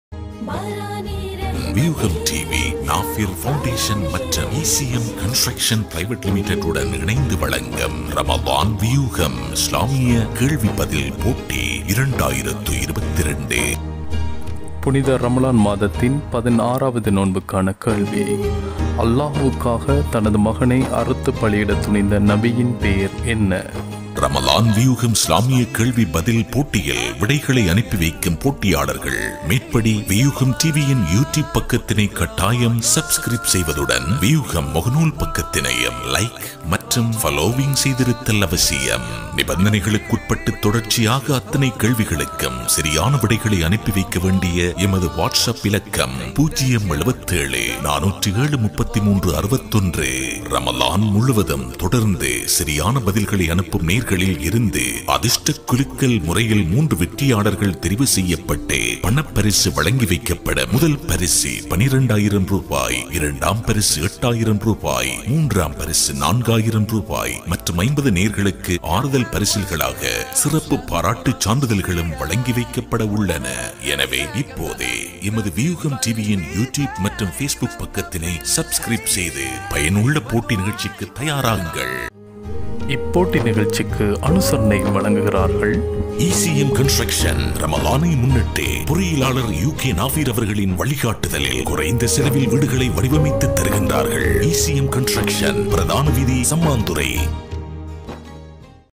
ரமழான்_வியூகம்| இஸ்லாமிய வினா விடை போட்டி- 2022| 18.04.2022 RamadanQuiz:No:16
ரமழான்_வியூகம்| இஸ்லாமிய வினா விடை போட்டி- 2022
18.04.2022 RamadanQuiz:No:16

தொடர்ச்சியாக சரியான விடைகளை அனுப்பி வைத்து ரமழான் இறுதியில் பெறுமதியான பணப்பரிசில்களை வெற்றி கொள்ளுங்கள்.

அதிகம் பகிருங்கள்.